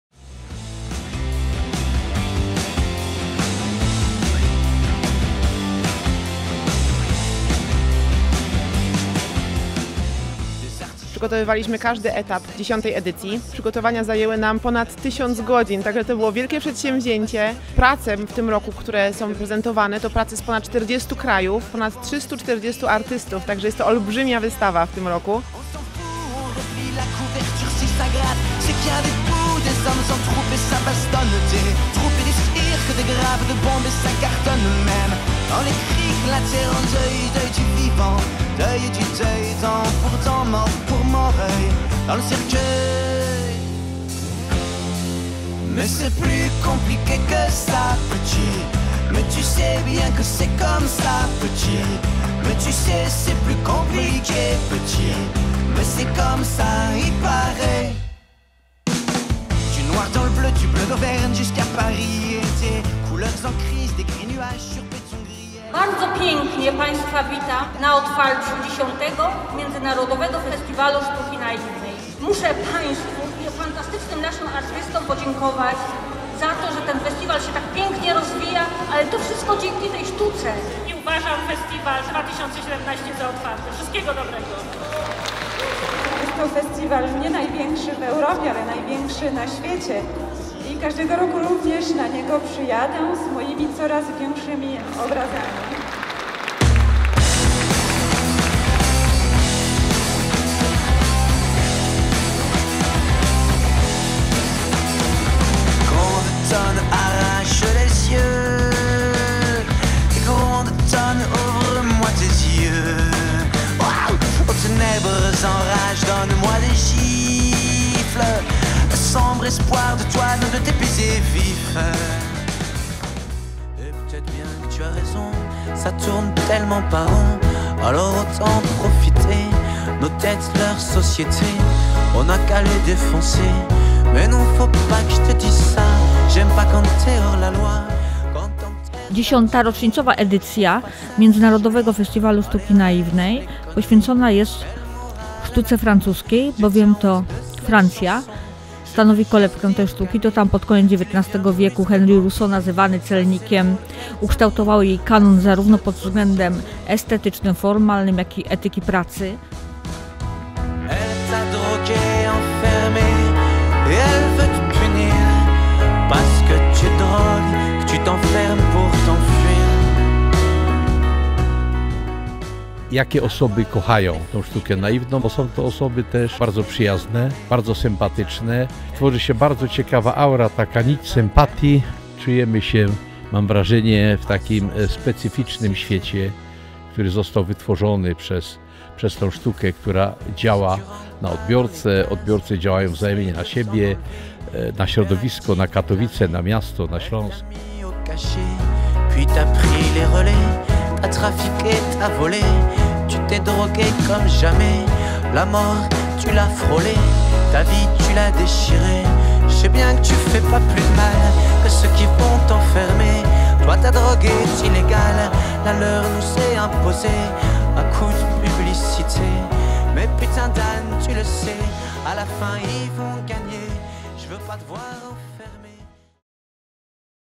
X Art Naif Festiwal (2017) - wernisaż
Jubileuszowa edycja Art Naif Festiwalu potrwa od 9 czerwca do 18 sierpnia 2017. Motywem przewodnim X Art Naif Festiwalu jest FRANCJA - kolebka sztuki naiwnej.